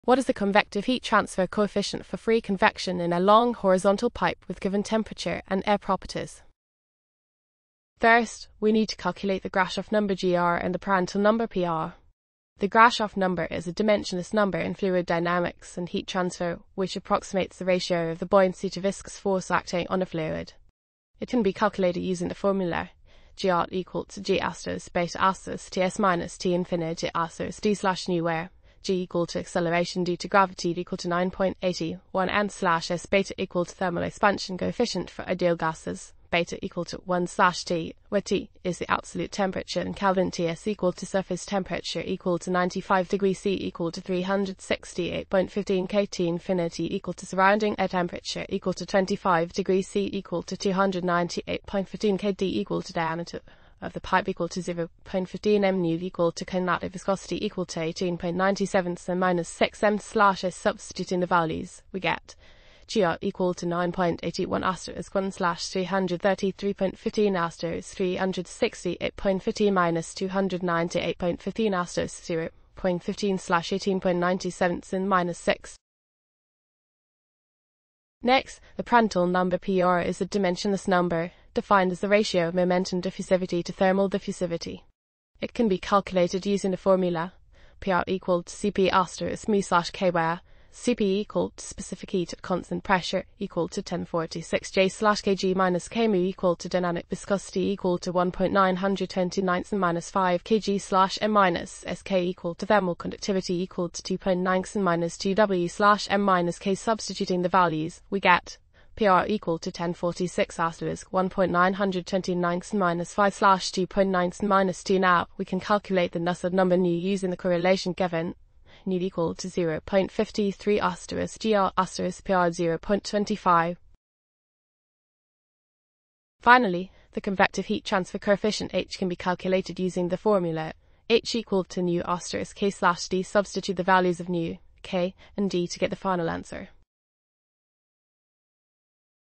A long horizontal pipe of 15 cm outside diameter passes through a large room The surface temperatur
A long horizontal pipe of 15 cm outside diameter passes through a large room. The surface temperature of the pipe is 95°C and the surrounding air Is at 25°C. Work out the convective heat transfer coefficient for free convection. Use the correlation: Nu = 0.53 (Gr.Pr)0.25. Take the air properties at mean film temperature of 60°C as : Cp = 1046 J/kg-K, k = 2.9×102 W/m-K, v = 18.97x106 m⅔, u=1.929x10-5 kg/m-s